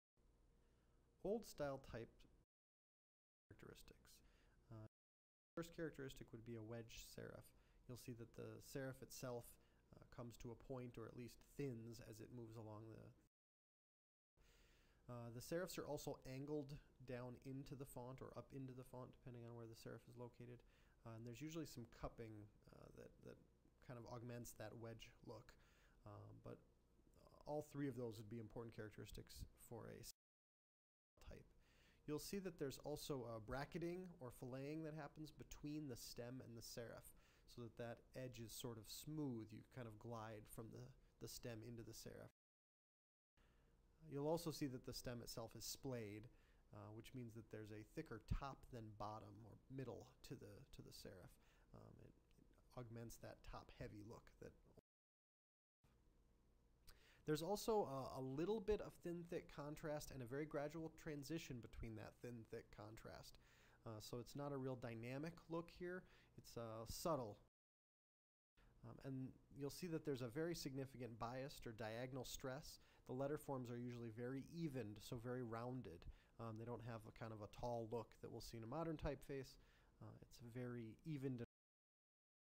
Old Style
A description and example of old style typefaces.